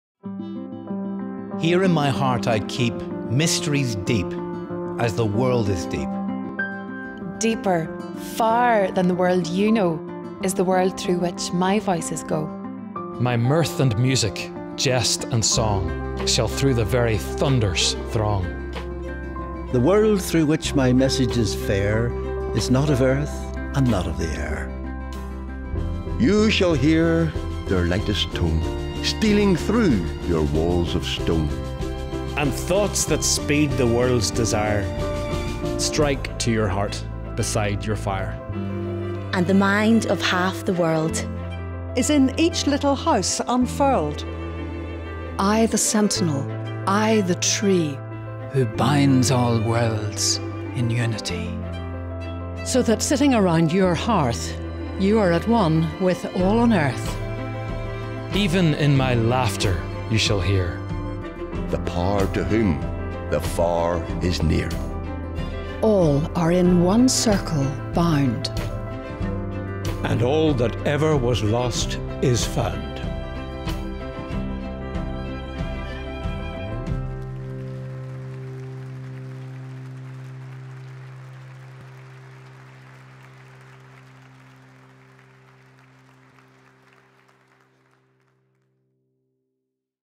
If you love RADIO let it show.
Filmed as part of the BBC Radio Ulster 40th Anniversary Gala Concert in the Ulster Hall, this film features BBC Radio Ulster presenters and contributors reciting an extract from the poem “The Dane Tree” by Alfred Noyes, a celebration of airwaves and the power of radio. The poem was originally read at the official opening of the Daventry Transmitting Station in 1925 – the transmitter allowed the BBC to broadcast radio content across the world, with what was then called The Empire Service.
Today it is known as The World Service.